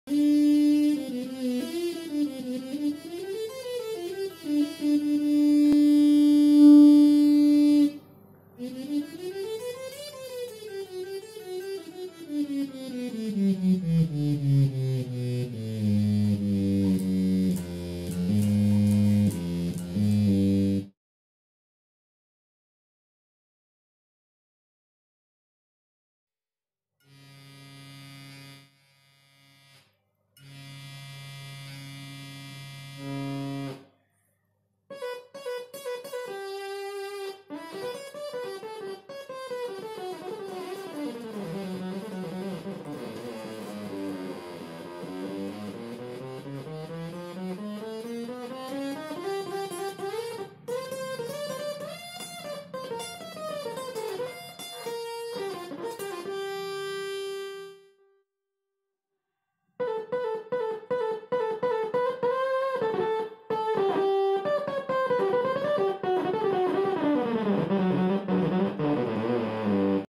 Korg X 911 mod to GR 300
In my opinion one of the most underrated vintages "toy" ever made .
The Korg X-911 modified here to have one of its
 internal sawtooth (that"s used only for the Hz/V control voltage extraction ) gated directly as audio event in an effort to simulate the famous fast tracking of  GR-300.
First part played normally (unmodified) and around 19 Sec it's clearly obvious
the difficulty of the circuit to track the five lowest notes on the fretboard.!!
This is due to a compromise  of the Korg circuit to "sacrifice" the speed of tracking
by always "blanking" the first two periods of every new note in an effort to extract the
best posible (valid) pitch CV with its in series sample/track  & holds and hold that value as clean as possible for a long duration .
By the way the Korg X-911 has the best  S&H (imho) in terms of droop by using  offset selected Mosfet CA3140 and polypropylene capacitors soldered
on the air as a means of quard ring !
The rest video shows how fast it can track after modded